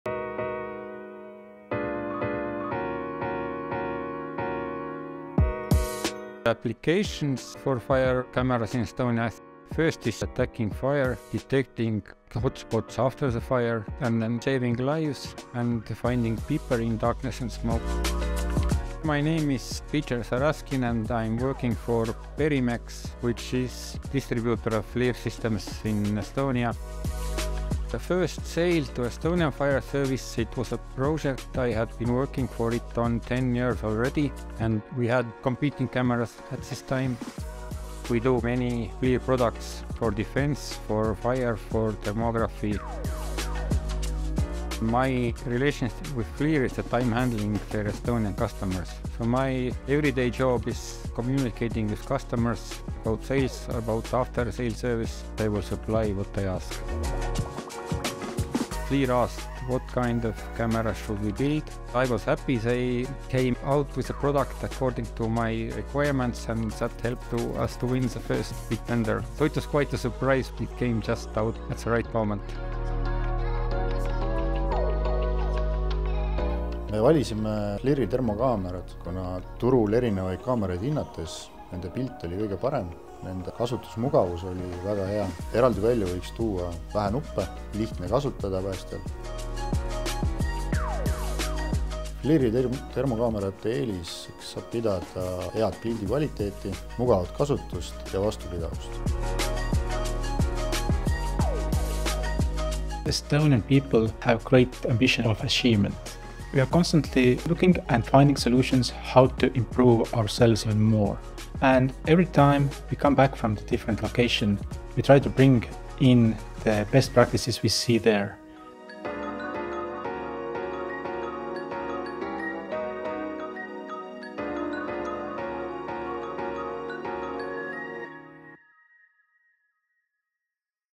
Working with Estonian Fire Service for 10 Years | Teledyne FLIR
In March 2013, FLIR (now Teledyne FLIR) released the first models in its K-series thermal imaging cameras for firefighting applications. A decade down the line, the ground-breaking K-series remains the market leader in firefighting applications, allowing crews to visualize a plan of attack, locate hot spots and save lives

Learn more about FLIR's decade of success with the FLIR K-series